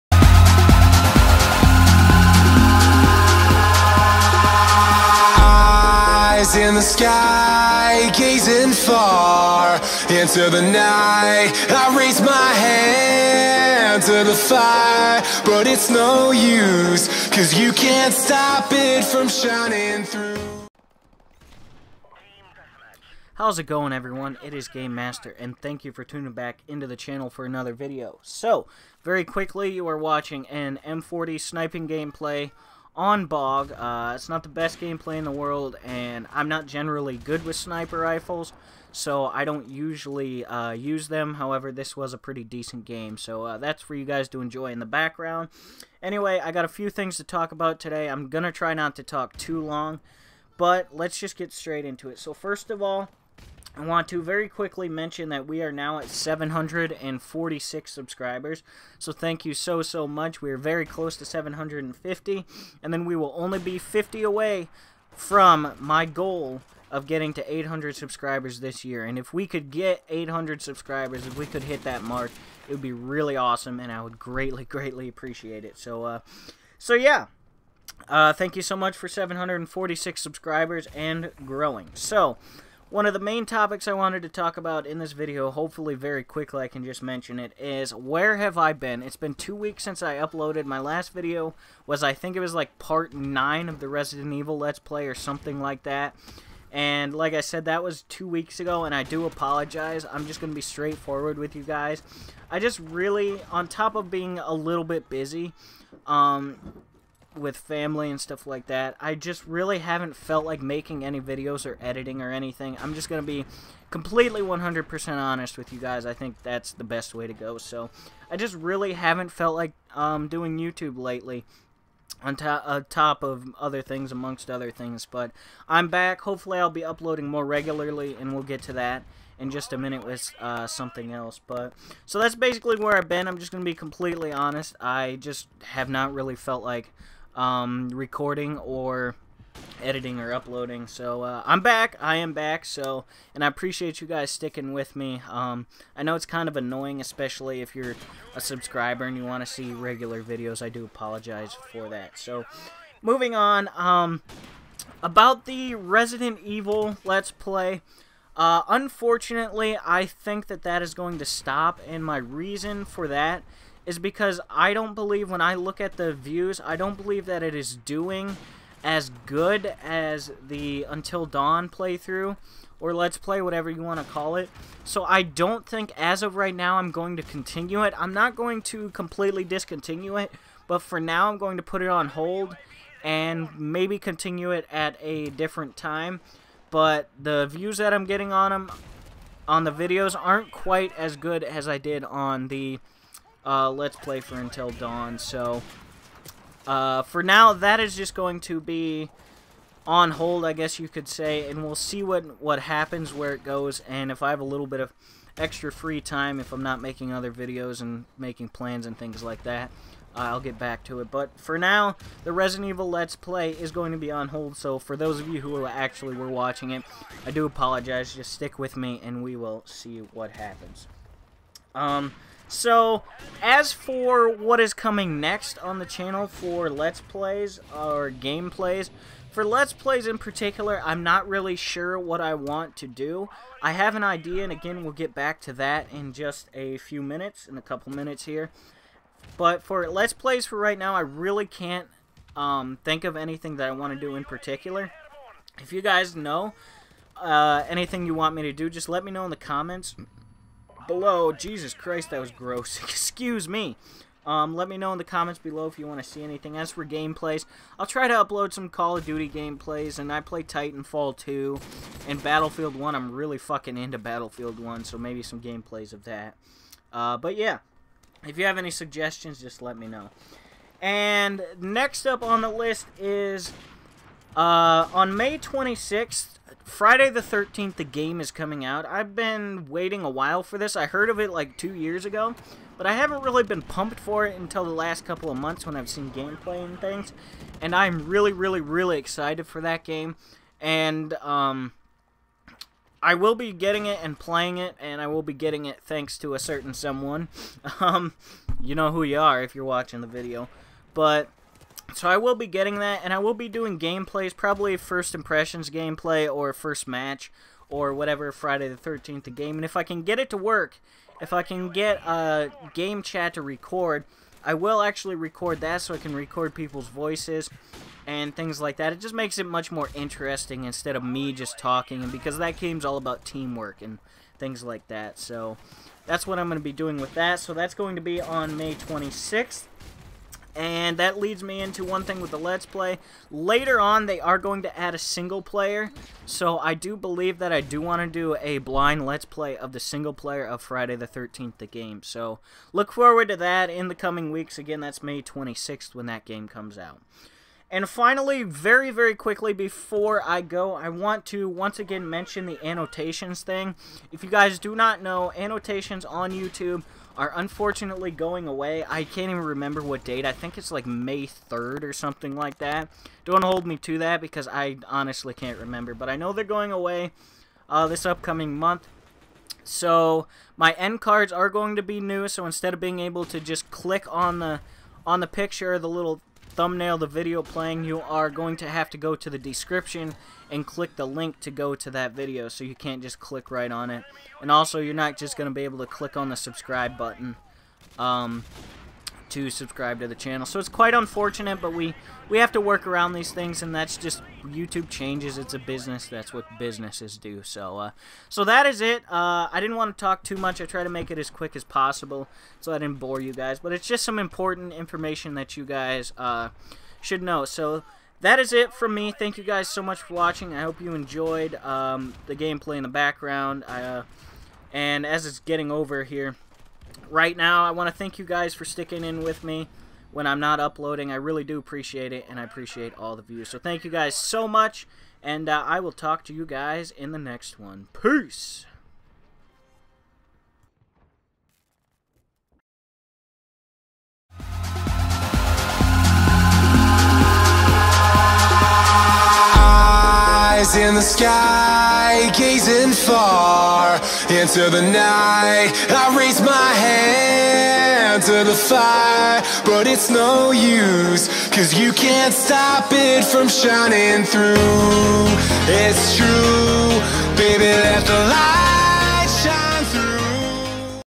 Where Was I? RE lets Play Update, F13 The game & More Channel Update 4 23 17
In this video we talk about a few thing regarding the channel.